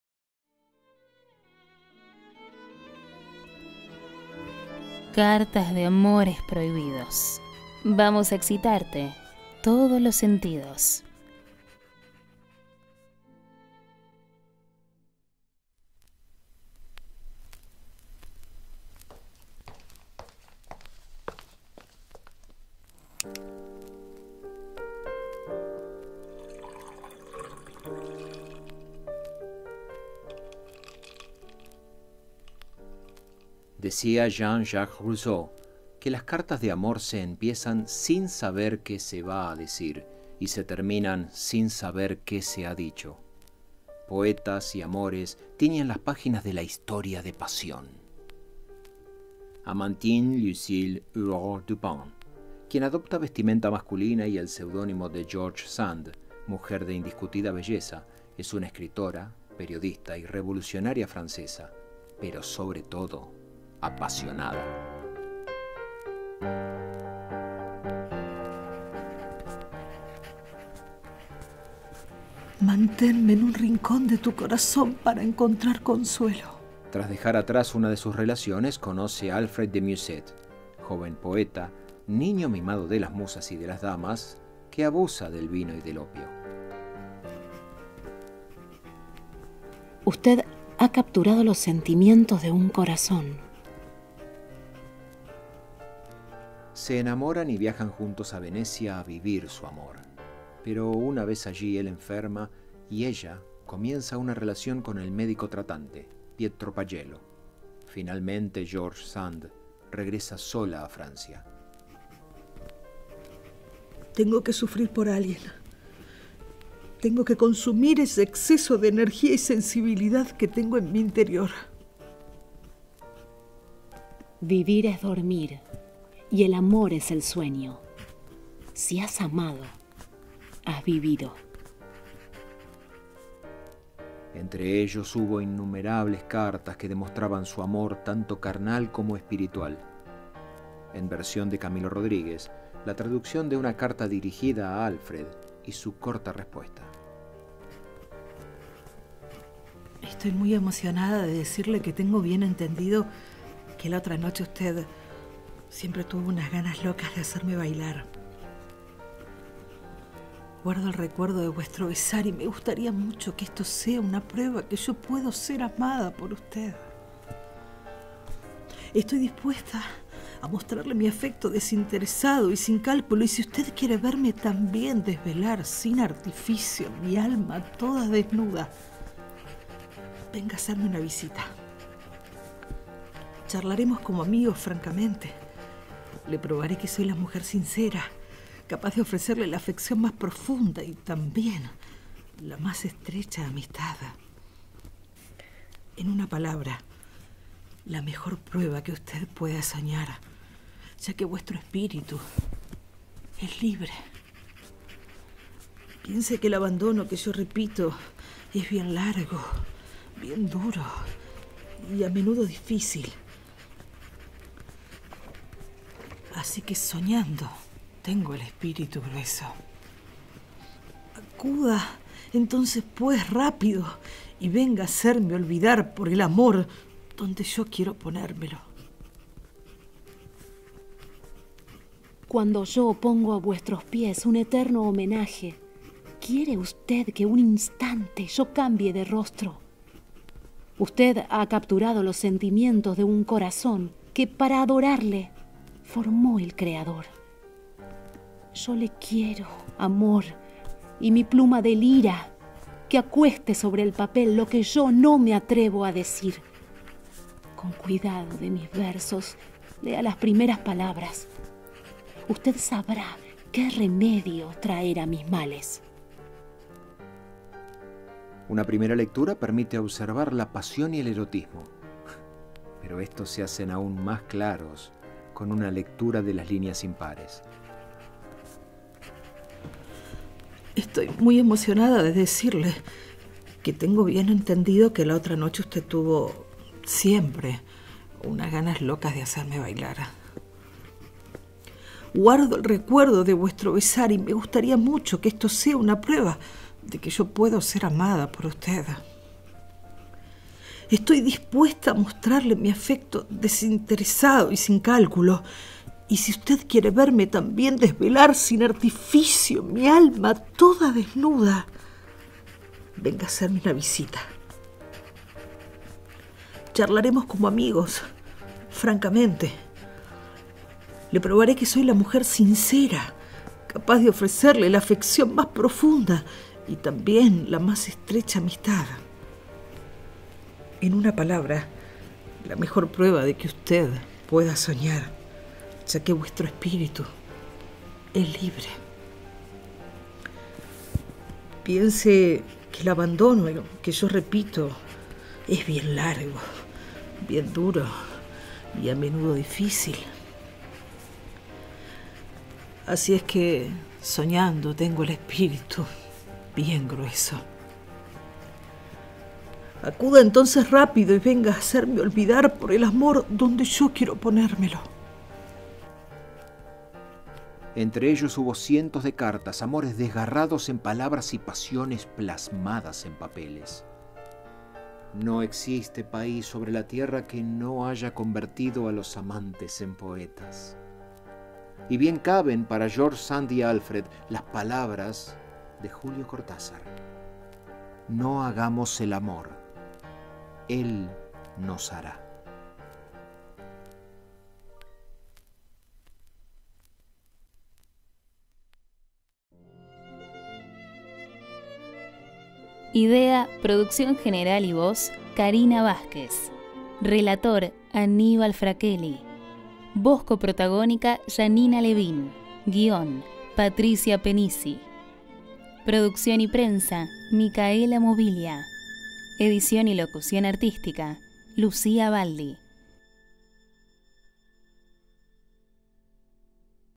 GEORGE SAND - ALFRED DE MUSSET
George Sand, Amantine Aurore Dupin, tal su verdadero nombre, fue una novelista y periodista francesa que se rebeló contra sus tiempos, no solo vistiéndose de hombre para dignificar su femineidad, sino al tener varios amantes famosos, entre ellos Alfred de Musset, escritor y dramaturgo francés.
Para ella el “no se puede” nunca fue una opción, y siempre consiguió lo que quería.
A través de sus cartas de amor prohibidas conocemos sus personalidades fogosas, indomables y de un contenido literario que va más allá de las letras; una pasión que Musset, seducido y abandonado, llevó hasta su muerte.